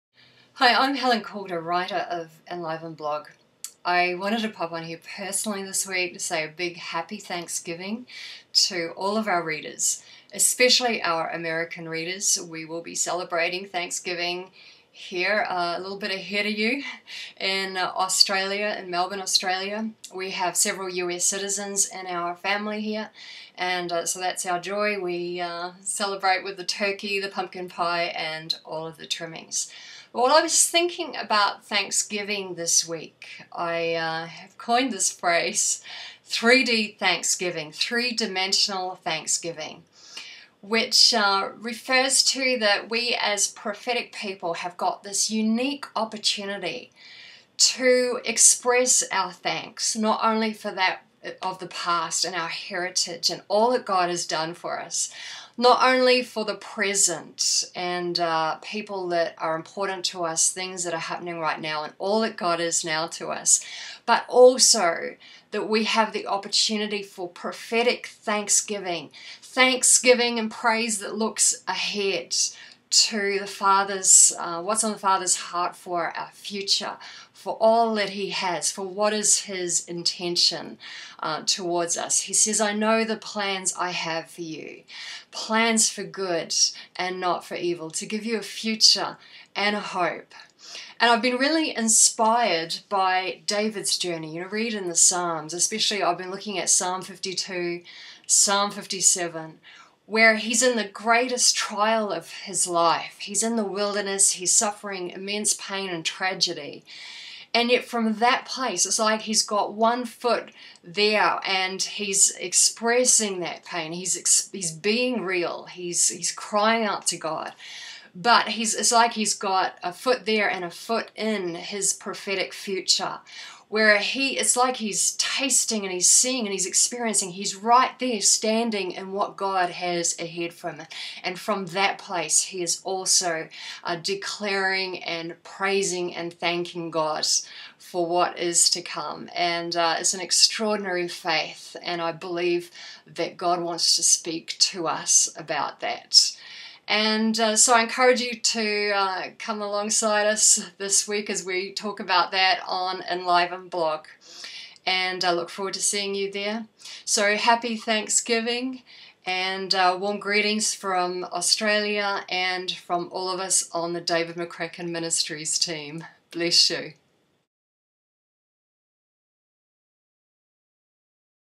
Prophetic Thanksgiving and Praise
Helen Calder shares an informal Thanksgiving message with readers of Enliven Blog with some thoughts about prophetic praise and thanksgiving.
Helen is a team member of David McCracken Ministries and author of Enliven Blog.